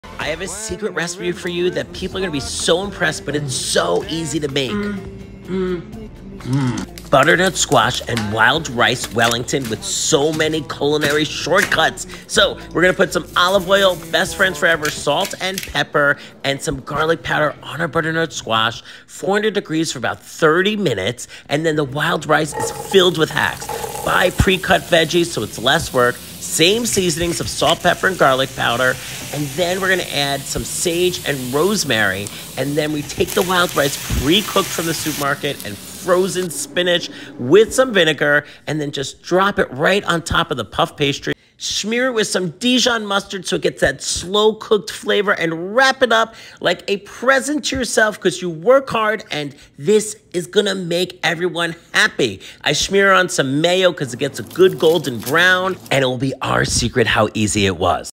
Butternut Squash Wellington Recipe
Butternut Squash Wellington is a perfect Fall recipe, Thanksgiving recipe, or Vegetarian holiday recipe.